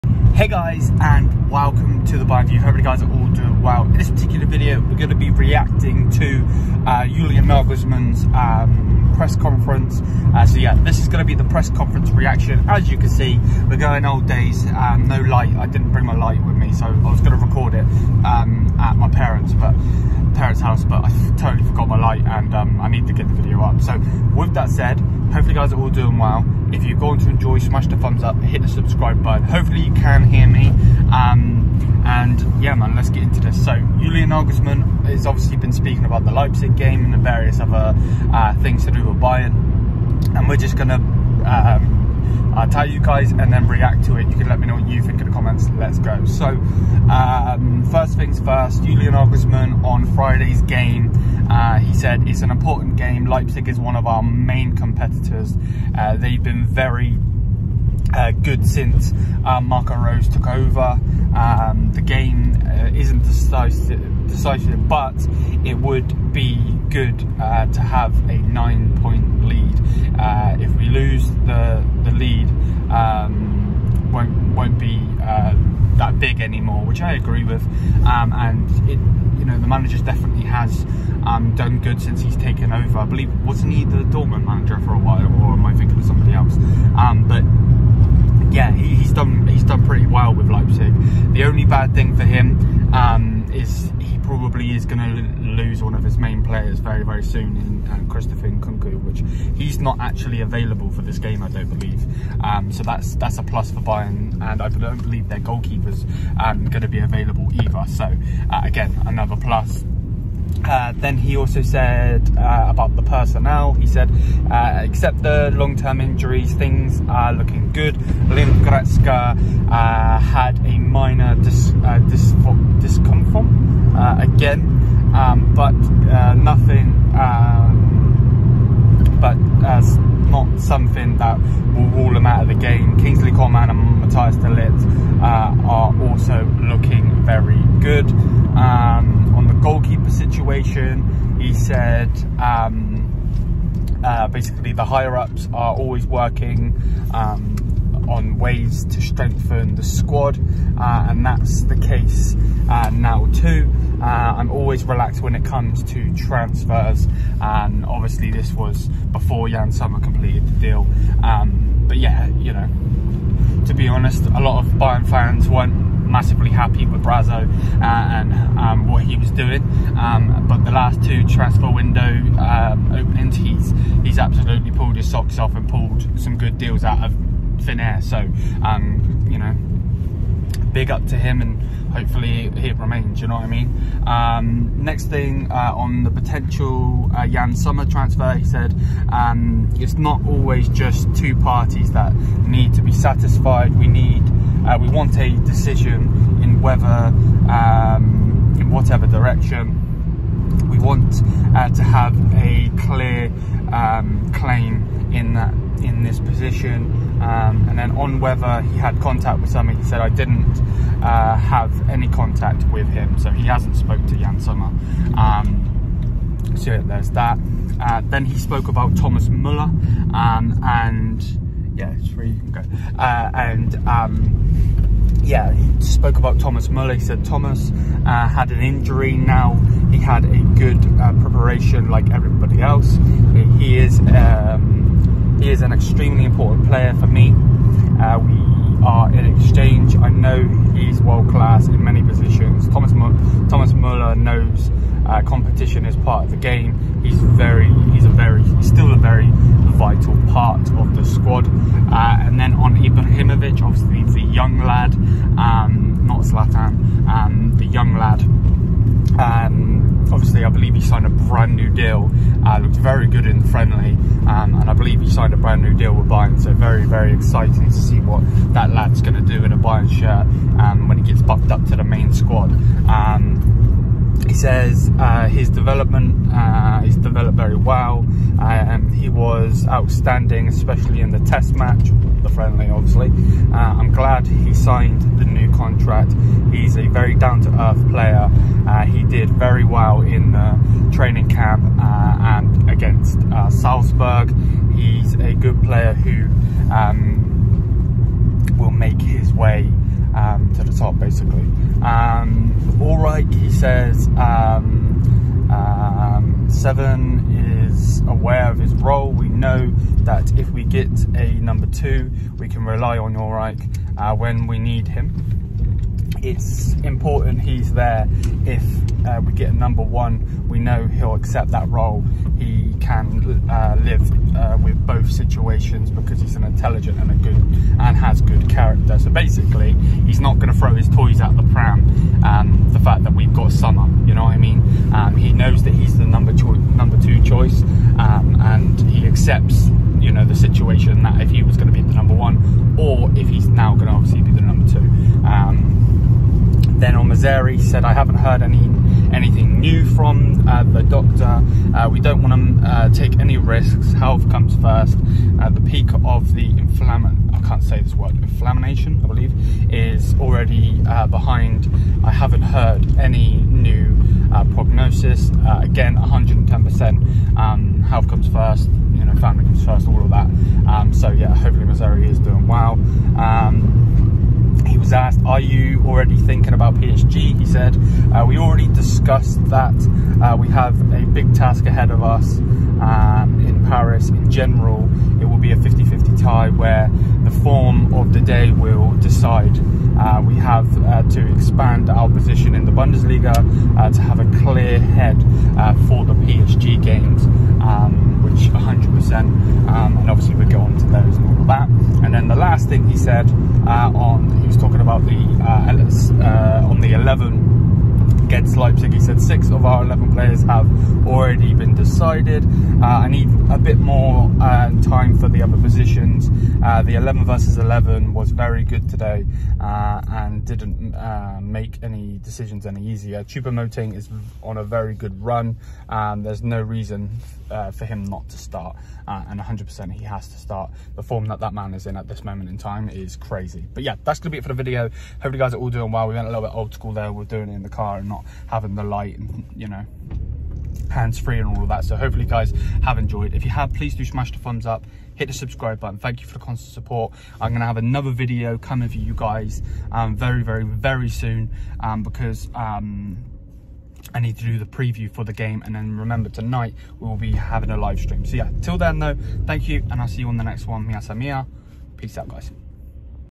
Julian Nagelsmann PRESS CONFERENCE Reaction! RB Leipzig Vs Bayern Munich - Bayern Munich News
Hi guys and welcome to The Bayern View! In todays video we discuss and react to Julian Nagelsmann PRESS CONFERENCE

Ignore ❌